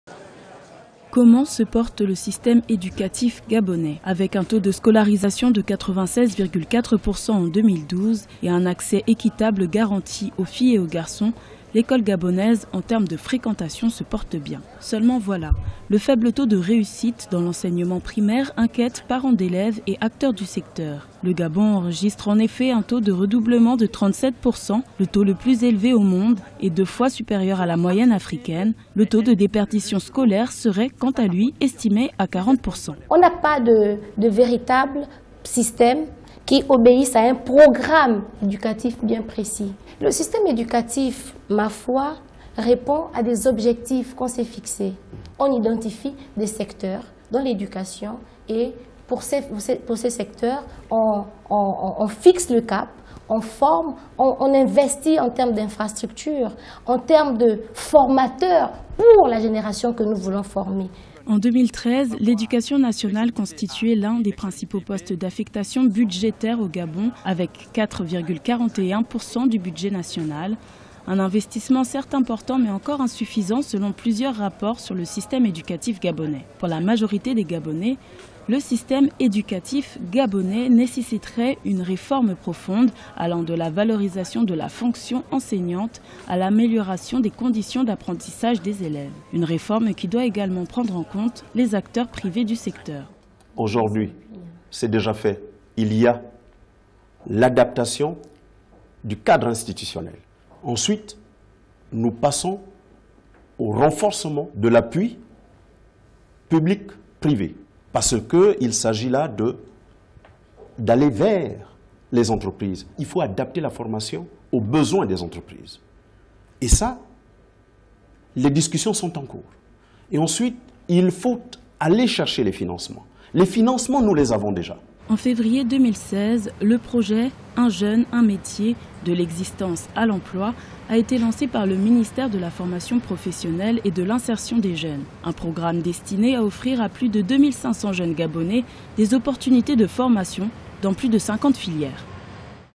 Gabon, L'accès à l'éducation
Société, GABON: Au Gabon, la situation de l’enseignement affiche un taux d’alphabétisation qui se situe autour de 90%, bien au-delà de la plupart des pays d’Afrique subsaharienne. Cependant, une analyse approfondie permet de démontrer que le tableau est bien moins reluisant.